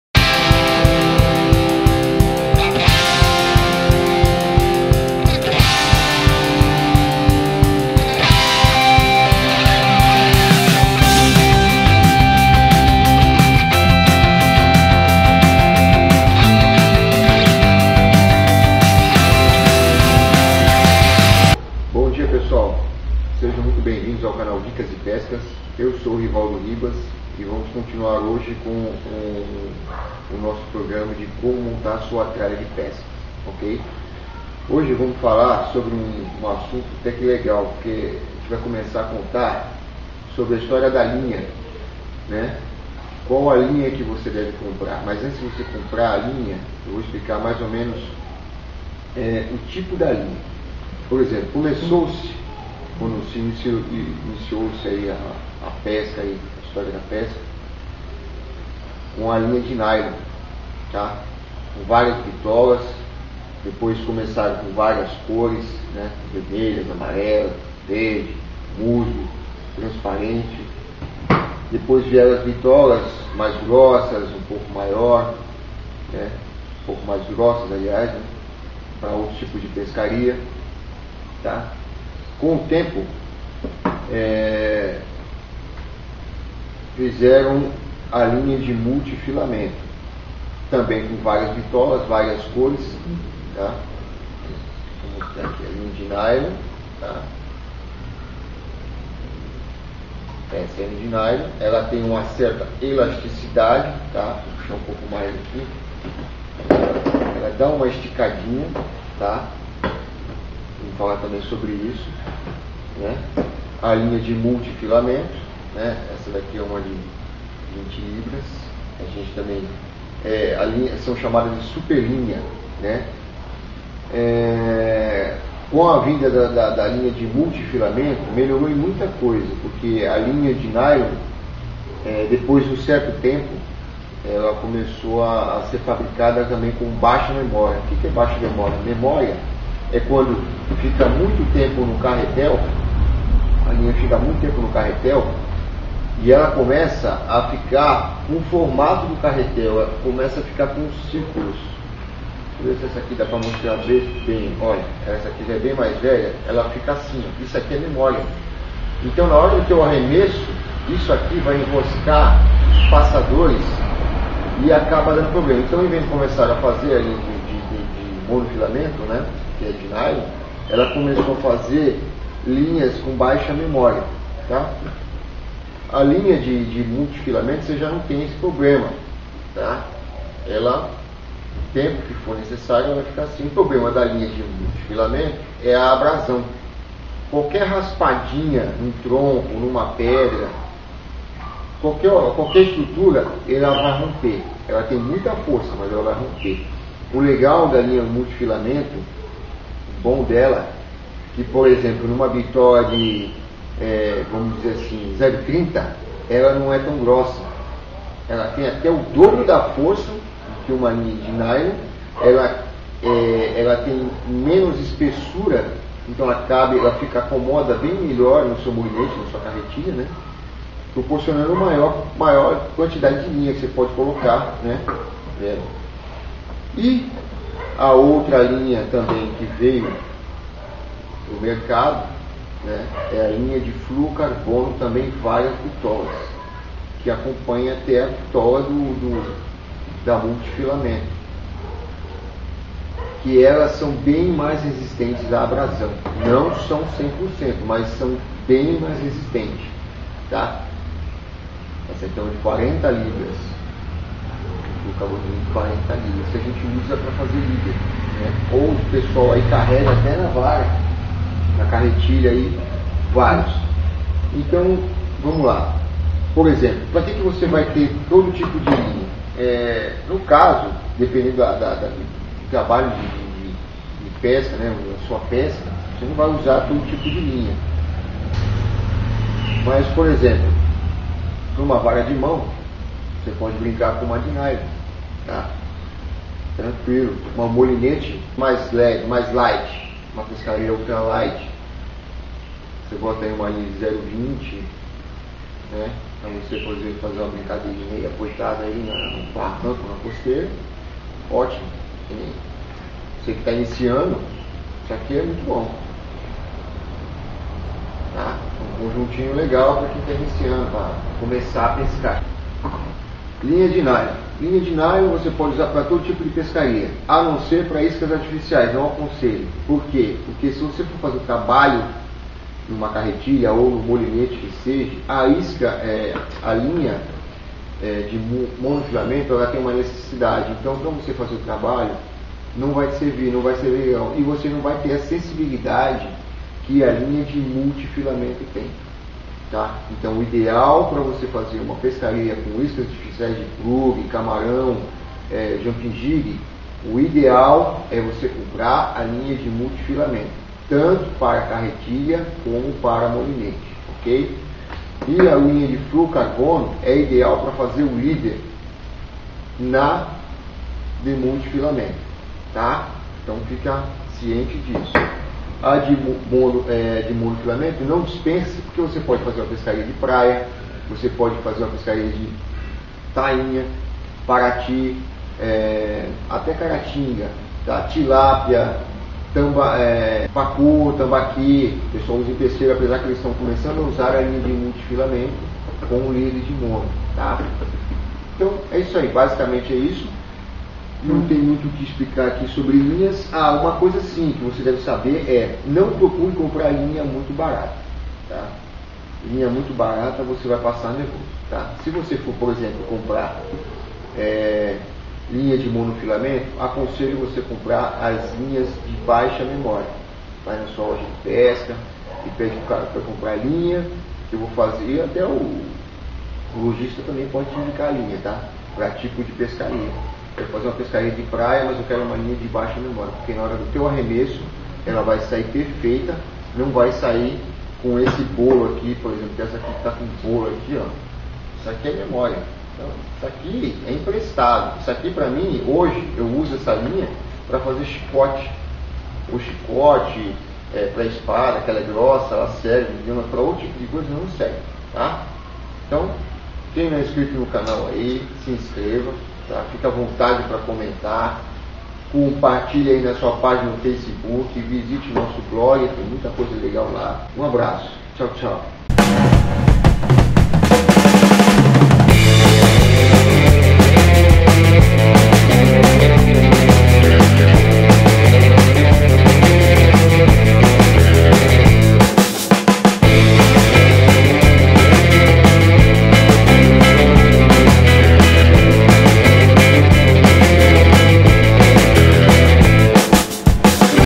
Como Montar Sua Tralha de Pesca - PGM#7 - Linhas de Pesca
Neste vídeo mostro as diferenças das linhas de pesca e para qual tipo de pesca elas servem